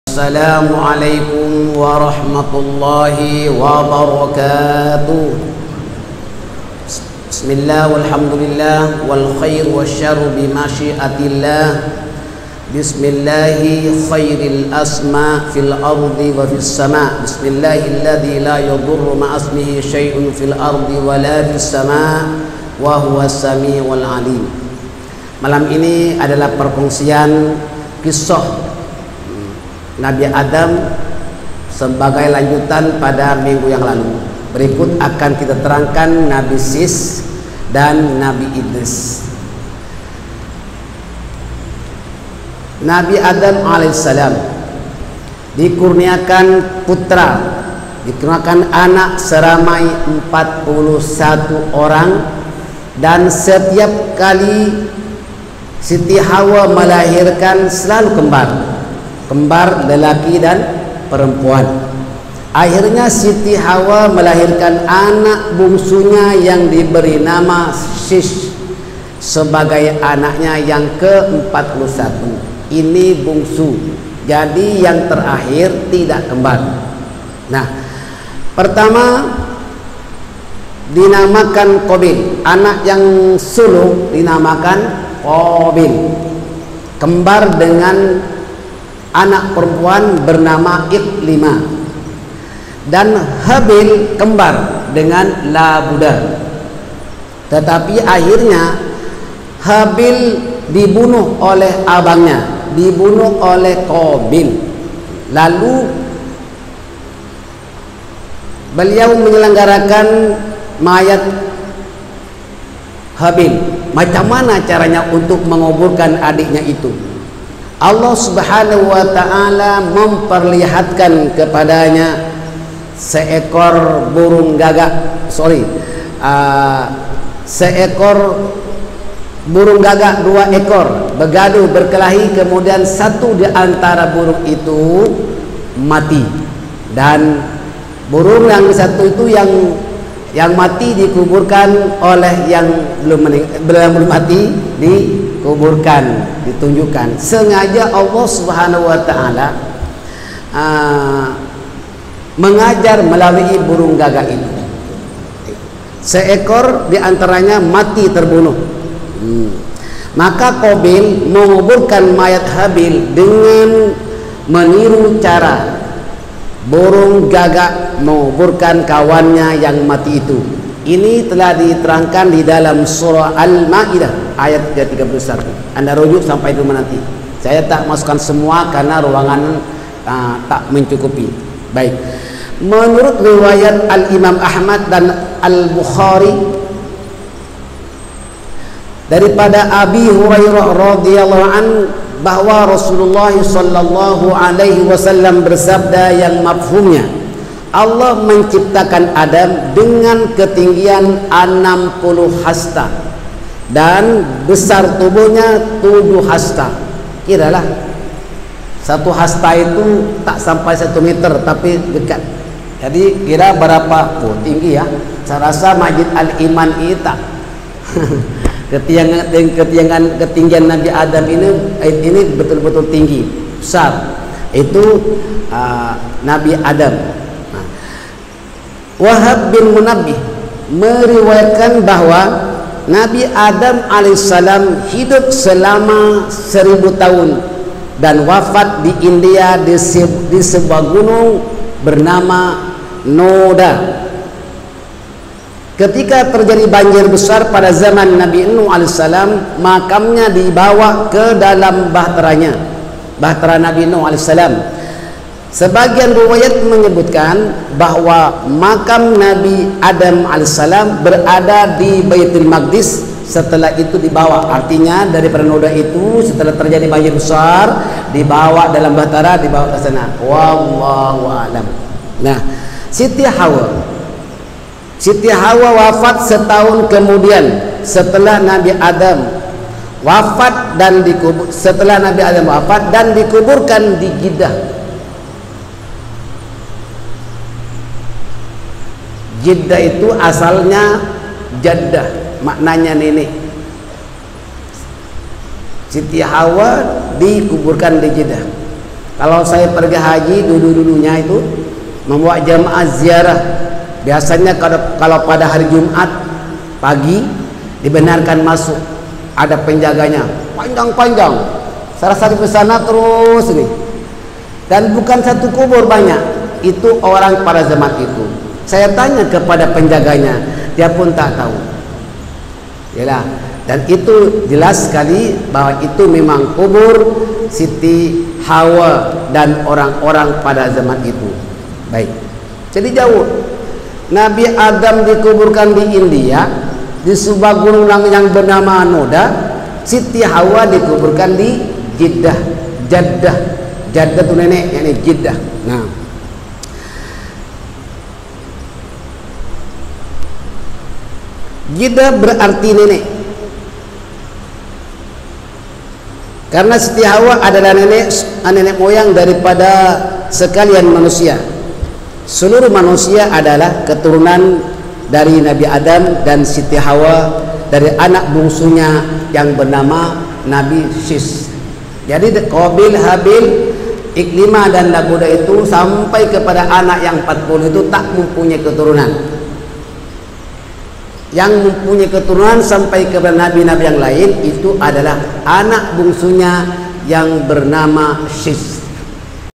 Zuriat Nabi Adam a.s. - Ustaz Drs HM Noor Tijany
Rujuk Kitab: Minhajul Mu’minin Jilid 3 (Ms 19)

Kelas mingguan pimpinan Ustaz Drs HM Noor Tijany @ Masjid Al-Iman, Singapura pada setiap hari Rabu

Semoga bermanfaat
Terimakasih

Tarikh rakaman: 12hb February 2020